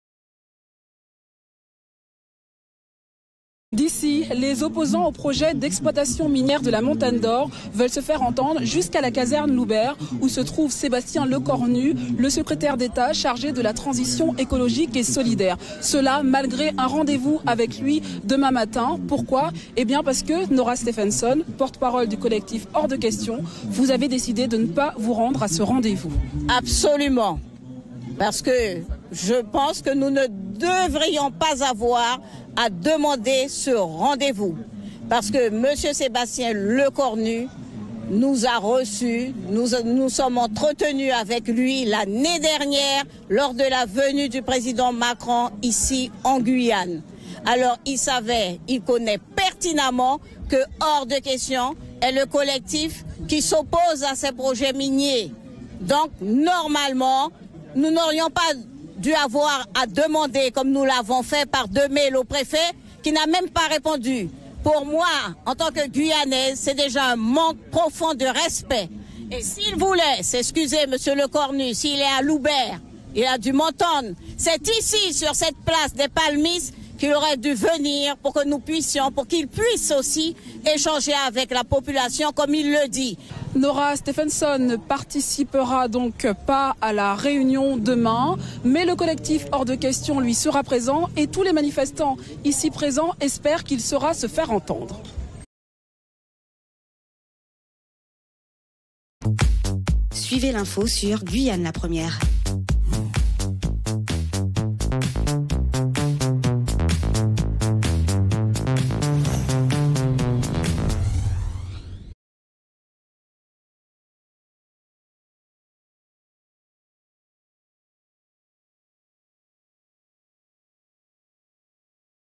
Manifestation du collectif Or de question devant la préfecture de Cayenne
Manifestation du collectif Or de question devant la préfecture de Cayenne. Nora Stéphenson la présidente du collectif a décidé de n'être pas présente au rendez-vous avec Sébatien Lecornu le 26 juillet au matin